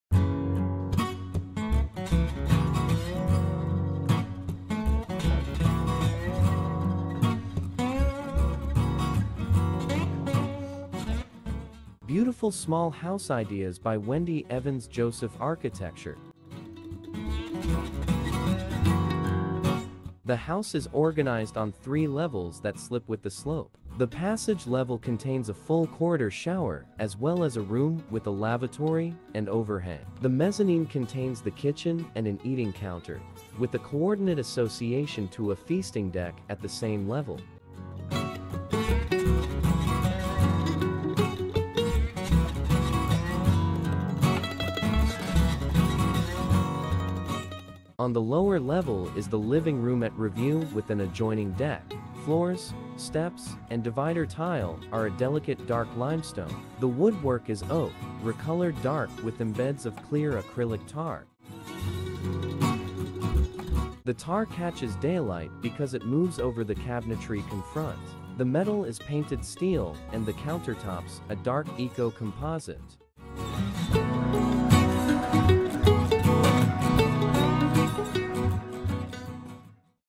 Beautiful Small House Ideas by Wendy Evans Joseph Architecture
This agricultural property in Sonoma is owned by two scientists. The husband often works inside, but also prefers alternative work locations where for his laptop computer. The wife is involved with a number of agricultural projects, keeping olive orchards, harvesting olives and making olive oil; bee keeping; extensive gardening and various other endeavors that take advantage of Sonoma’s beautiful climate, soils and siting. Embracing the “locavore” movement, the wife brings her olives and honey to local markets and restaurants.

The 25-acre site of the original house had no real infrastructure and met none of the new code requirements for water conservation and fire safety, and did not provide adequate power for farm use. The client requested a new structure that would take advantage of the site’s hillside views, while reflecting their modest lifestyle so deeply engaged with outdoor life. They also requested that the new structure be “off the grid”, with all utilities buried, and that site improvements be implemented to avoid erosion.

The house, only 850 sqf, is situated towards the top of an olive orchard. The siting takes advantage of the prevailing breezes and shade, while utilizing an existing open area so that no trees were removed. The goal was to create an energy efficient, compact dwelling where the clients could sit in shady areas, entertain, and enjoy views across the local agricultural lands.

The house is organized on three levels that descend with the hillside. The entry level contains a full hallway bath as well as a bedroom with a bathroom and balcony. The mezzanine contains the kitchen and an eating counter, with a direct connection to a dining deck at the same level. On the lower level is the living room at grade with an adjacent deck. Floors, steps, and wall tile are a soft grey limestone. The woodwork is oak, stained grey with inserts of clear acrylic resin. The resin catches sunlight as it moves across the cabinetry face. The metal is painted steel and the countertops a grey eco-composite.

The exterior is predominantly light-grey zinc expressed in horizontal panels with 3/8” joints running horizontally and overlapping joints in the vertical direction. There are some minimal areas of redwood, stained grey as a detail on the south and east facades. The window system is custom, with steel mullions painted grey to match the zinc. Slatted, redwood sunscreens for the bedroom and entry deck provide texture, privacy, and shading. Horizontal skylights over the stair bring daylight to the interior while mitigating heat gain with a ceramic frit.

Furnishings are mostly custom built-in pieces as the room dimensions are small, and space must be used in a highly efficient manner. A more luxurious, large shower with 10-foot high glass overlooking the hillside, celebrates the view, and brings the outside indoors as a part of the residents’ functional lives.

The house is at once grand and intimate. We concentrated the limited square footage in a few select areas so that each actually seems quite generous. There are openings between levels so that one is able to converse easily and experience the view from a series of places within the house, from a secret window seat with a sliver window to the north to the large expansive view from the living room. Details like hidden closets and storage recesses are meant to delight the user with the confluence of efficiency and aesthetics.

UNITED STATES
Architects: Wendy Evans Joseph Architecture
Area: 850 ft²
Year: 2010
Photographs:Elliot Kaufman Photography